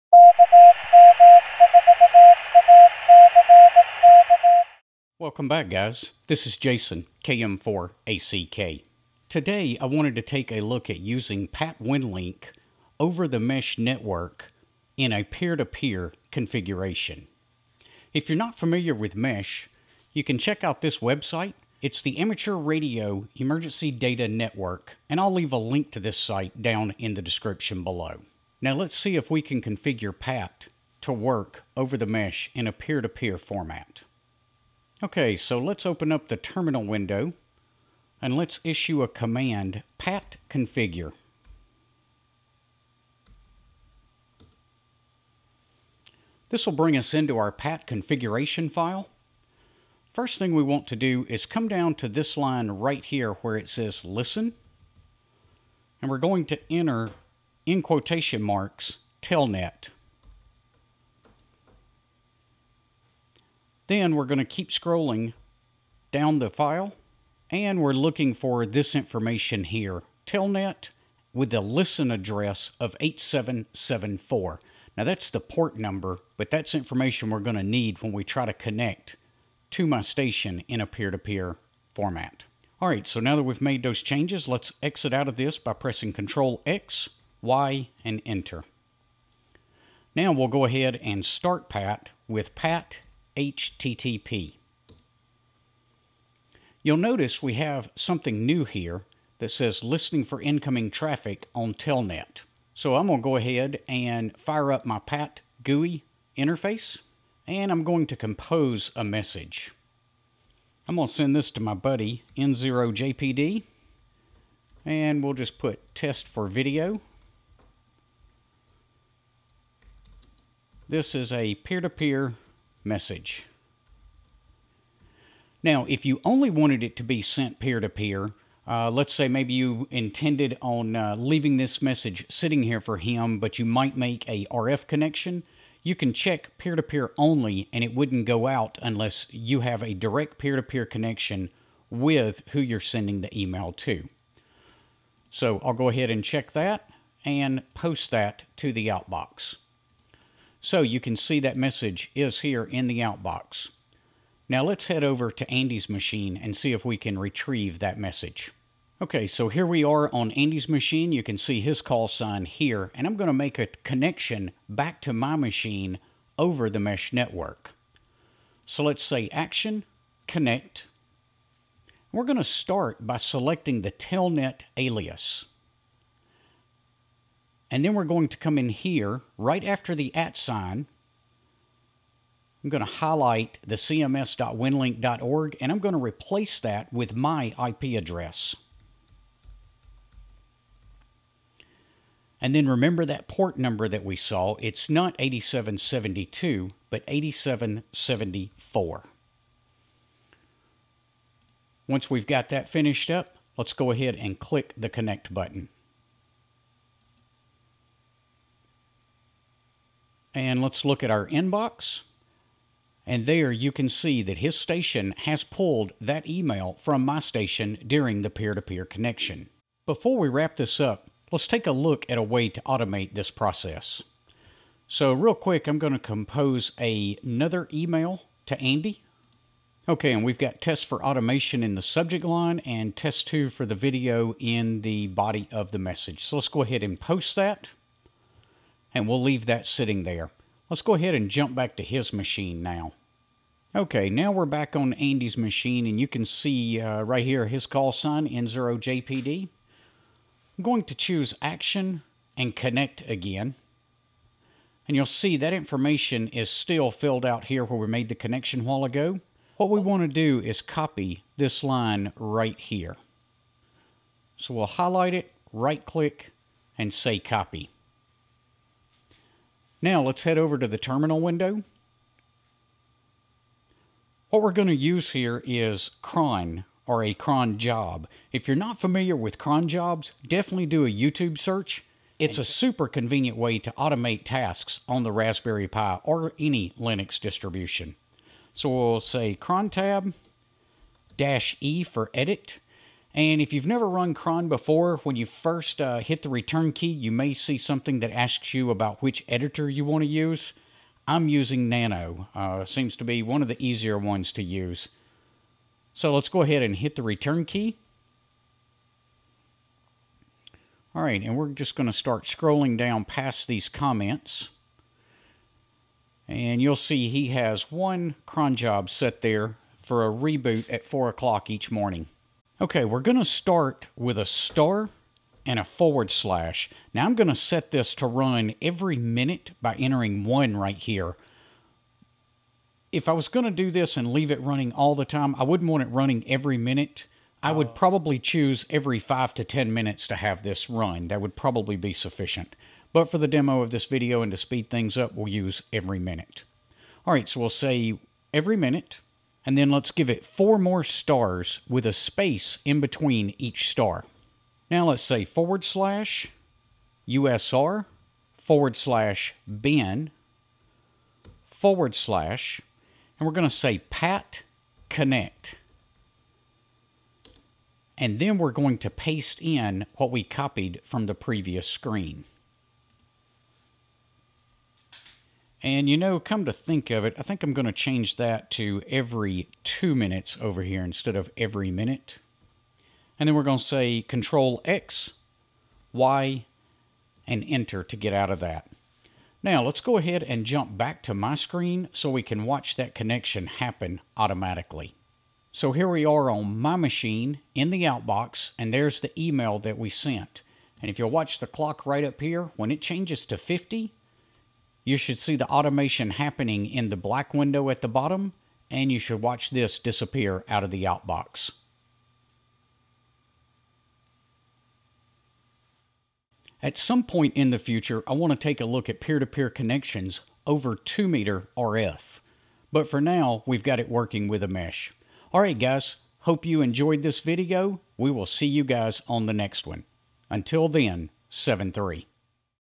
Winlink Peer to Peer over AREDN Mesh Network
How to run Pat Winlink over an AREDN mesh network for peer to peer connection.

73,
KM4ACK